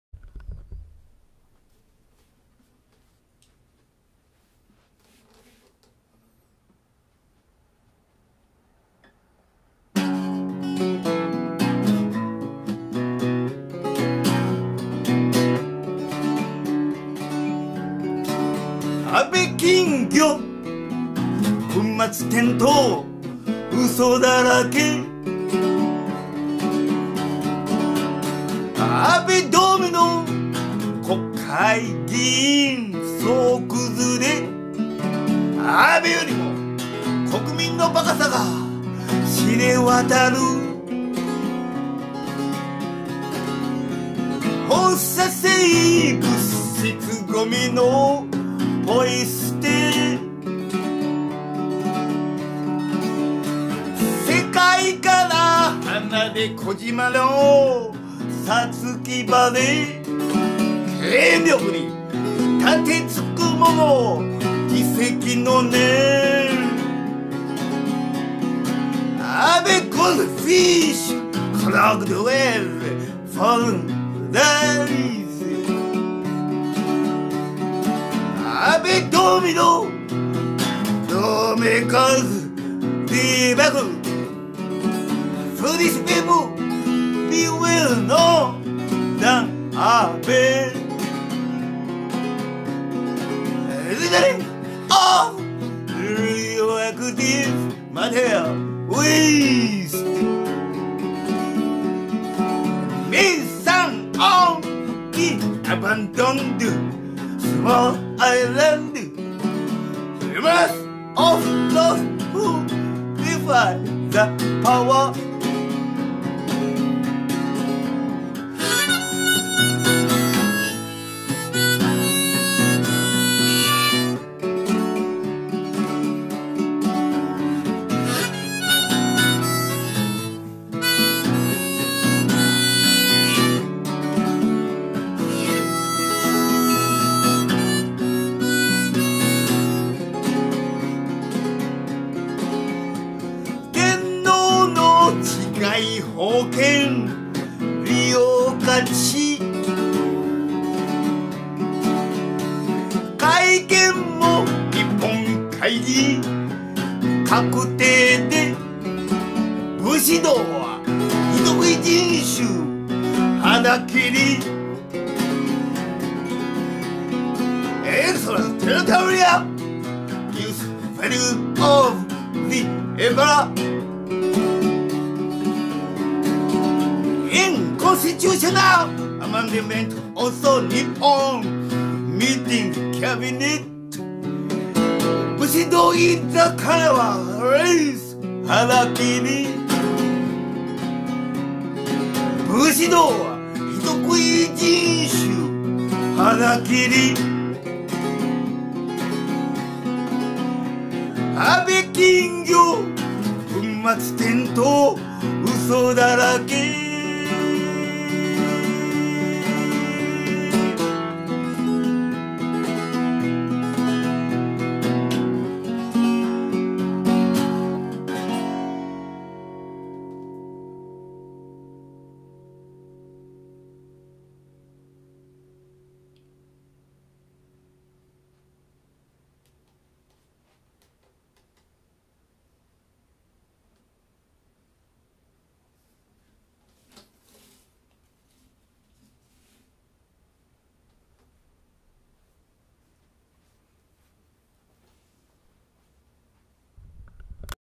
Harakiri by hisao
安倍金魚
糞末転倒
嘘だらけ

安倍ドミノ
国会議員
総クズれ！
 安倍よりも 国民の馬鹿さが 知れ渡る

放射性
物質ゴミの
ポイ捨て

世界から
離れ小島の
さつきバレ
権力に 楯突く者の 自責の念

Abe goldfish
clogged with
fallen lies

Abe Domino
law makers
debacle!
Foolish people be well-known than Abe

Littering of
radioactive -
material waste

May sun on
the abandoned
small island
Remorse of those who defy the power

天皇の
治外法権
利用価値

改憲も
日本会議
閣定で
武士道は 人食い人種 腹切り

Extraterritorial
use value of
the Emperor

In constitutional
amendment also
Nippon Meeting Cabinet
Bushido is a cannibal race harakiri

安倍金魚
糞末転倒
嘘だらけ